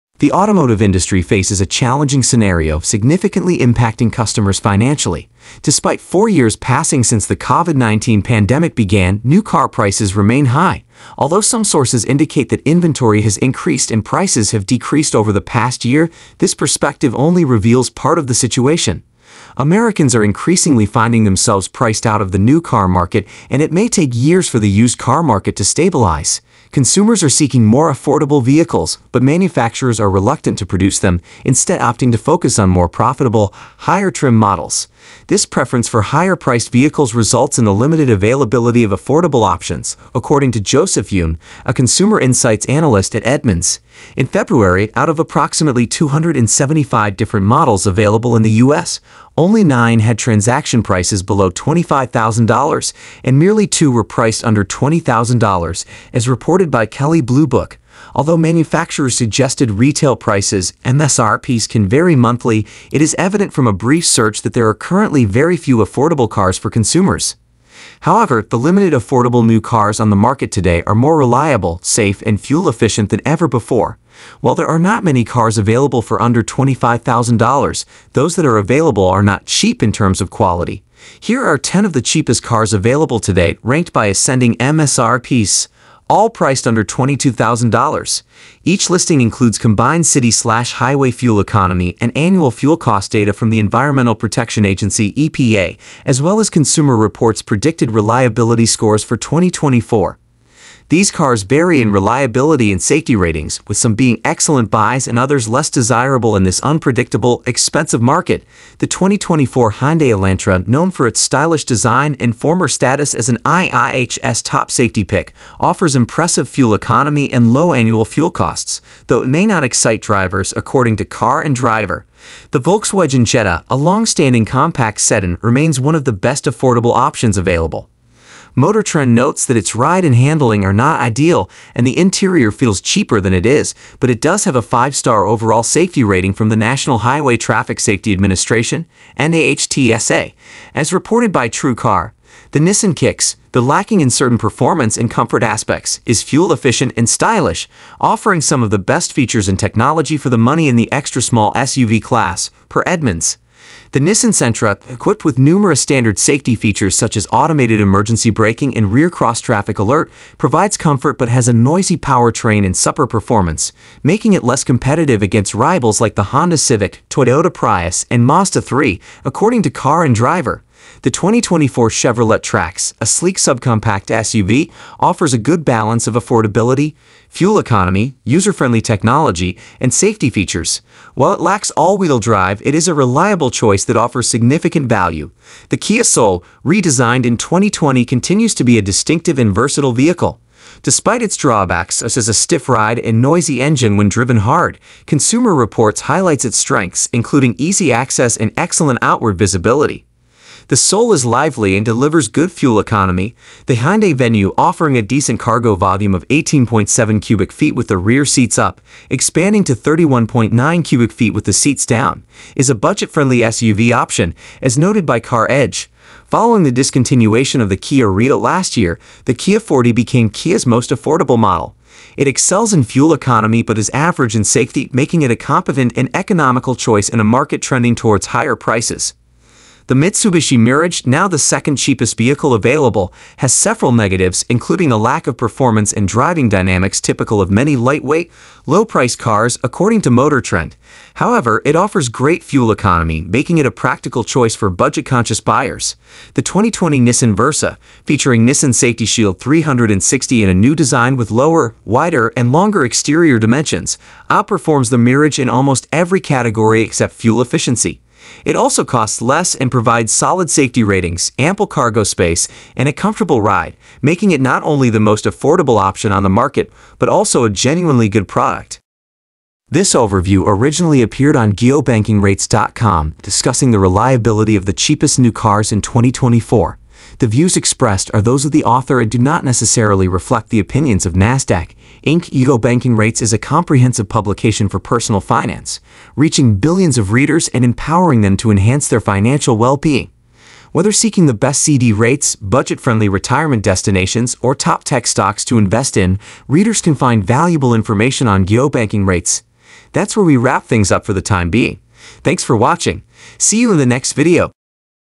Most Efficient And Cheapest Cars for 2024 | Specs, prices
Americans are increasingly finding themselves priced out of the new car market, and it may take years for the used car market to stabilize. Consumers are seeking more affordable vehicles, but manufacturers are reluctant to produce them, instead opting to focus on more profitable, higher-trim models. This preference for higher-priced vehicles results in the limited availability of affordable options, according to Joseph Yoon, a consumer insights analyst at Edmunds.In February, out of approximately 275 different models available in the U.S., only nine had transaction prices below $25,000, and merely two were priced under $20,000, as reported by Kelley Blue Book. Although Manufacturer’s Suggested Retail Prices (M.S.R.Ps) can vary monthly, it is evident from a brief search that there are currently very few affordable cars for consumers.

However, the limited affordable new cars on the market today are more reliable, safe, and fuel-efficient than ever before. While there are not many cars available for under $25,000, those that are available are not "cheap" in terms of quality.Here are ten of the cheapest cars available today, ranked by ascending M.S.R.Ps (all priced under $22,000). Each listing includes combined city/highway fuel economy and annual fuel cost data from the Environmental Protection Agency (E.P.A), as well as Consumer Reports’ predicted reliability scores for 2024.

These cars vary in reliability and safety ratings, with some being excellent buys and others less desirable in this unpredictable, expensive market.